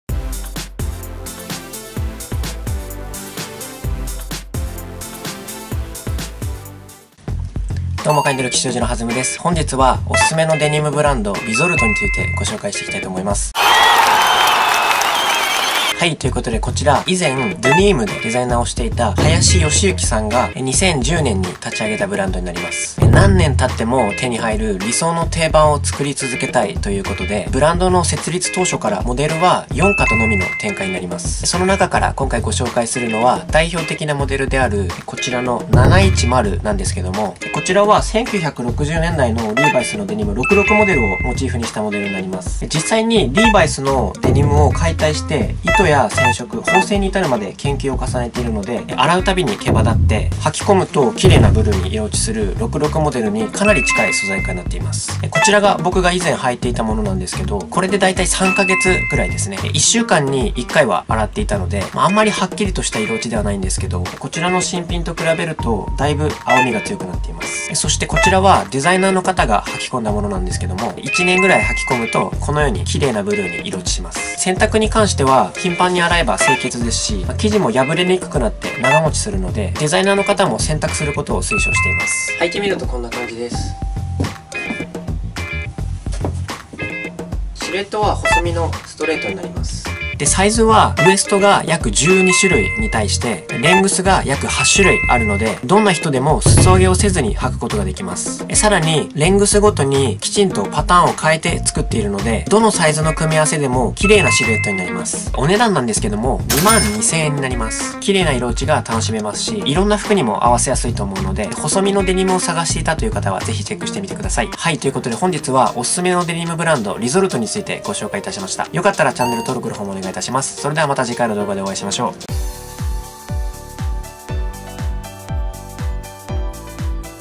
【デニム】鮮やかな色落ち！どんな体形でもシュッと決まるデニムブランド
こちらの動画の字幕作成にご協力いただける方いらっしゃいましたら宜しくお願い致します。ttp://

RESOLUTE【リゾルト】
下記URLでご購入できます！サイズの選び方やデザイナーのインタビューなども掲載されていますので是非！

DANのオンラインショップはこちらから